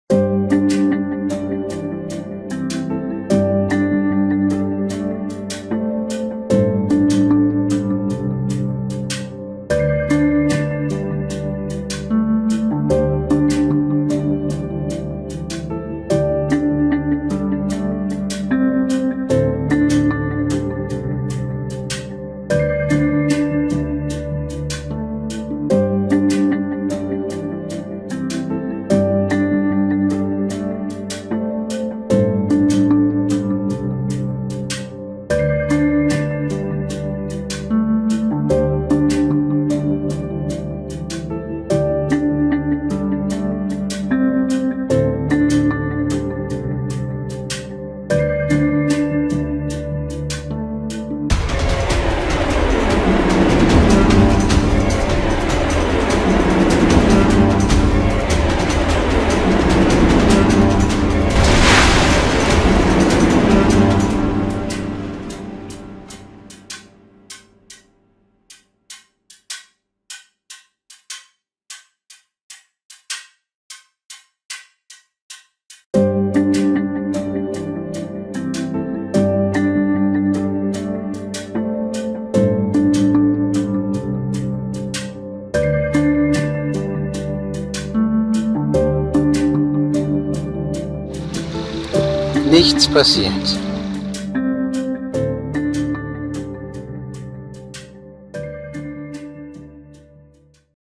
DJI Mavic Mini gerät ausser Kontrolle - der Zufall rettet sie
Gleich beim zweiten Ausflug macht sich meine neue Mavic Mini aus (mir) unerfindlichem Grund selbstständig - natürlich über Wasser.
Aus ebenso unerfindlichem Grund (vermutlich war es einfach nur Glück) rettet sie sich ans gegenüberliegende Ufer...